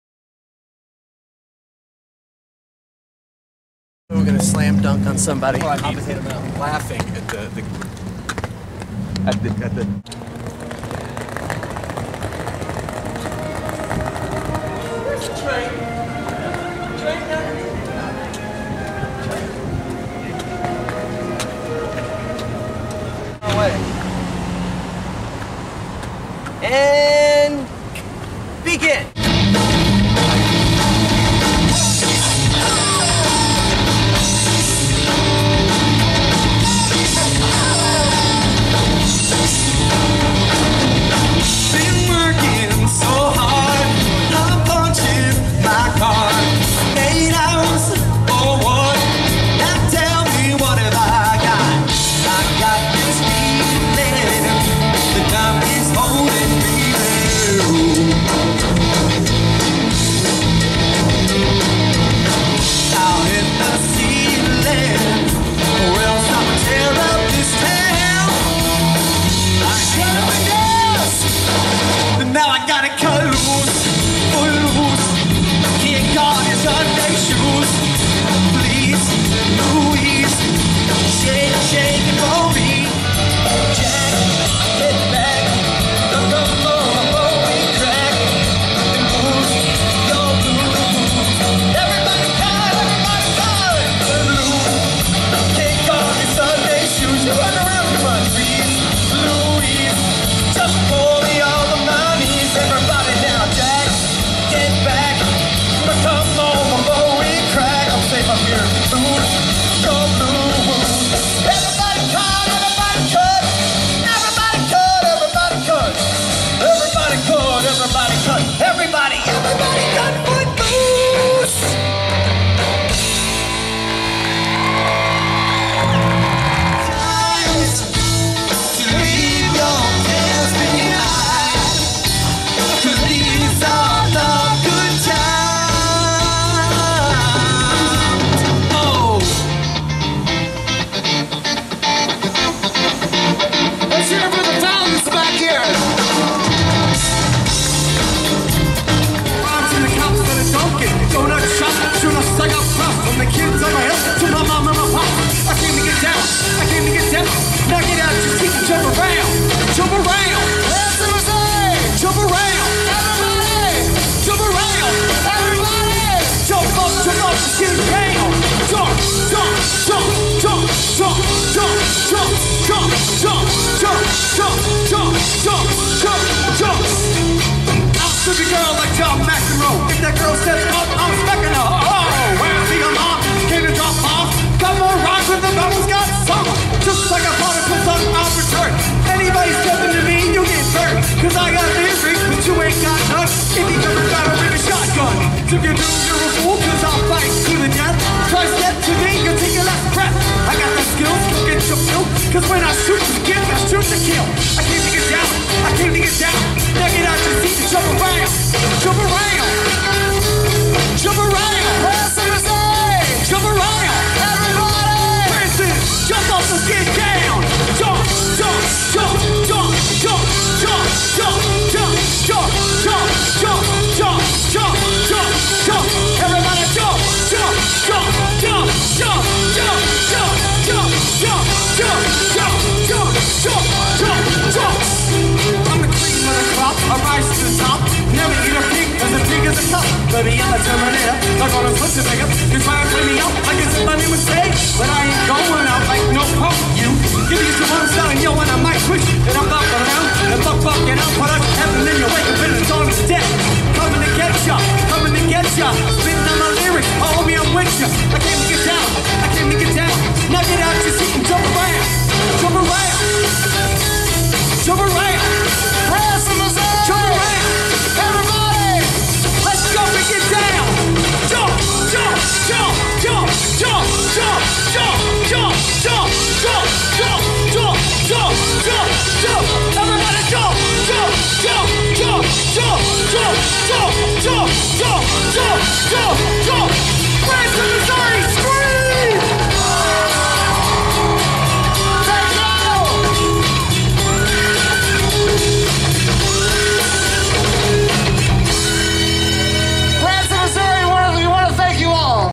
NAKED KARATE GIRLS Branson MO
Naked Karate Girls plays the Branson Landing in Branson MO. Great evening in for a concert in front of an amazing water fountain.